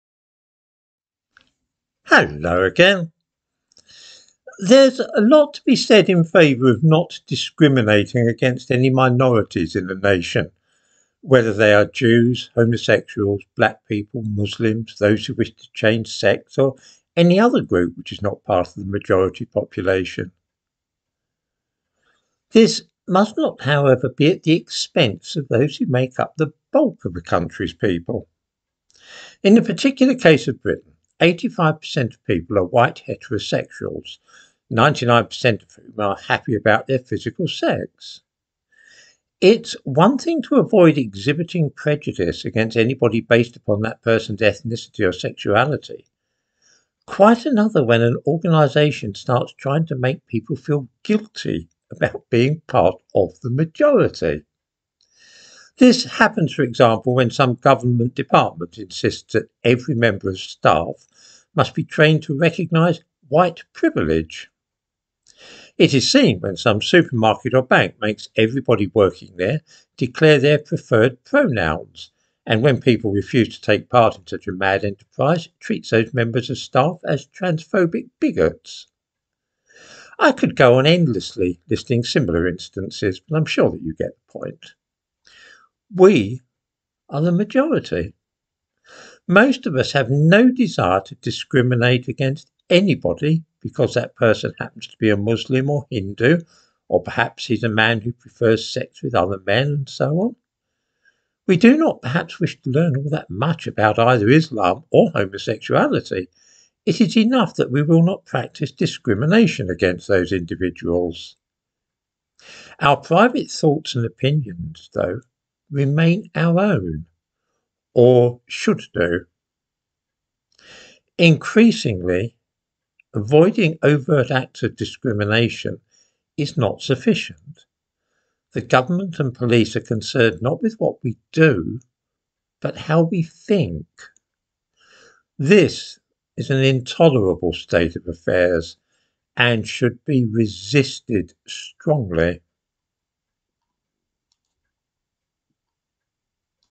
The rights of the minority should be protected, but what about the rights of the majority?
In Britain, we have bent over backwards to accommodate and, pander to, the wishes of every minority. We have however neglected to consider adequately the rights of the majority population.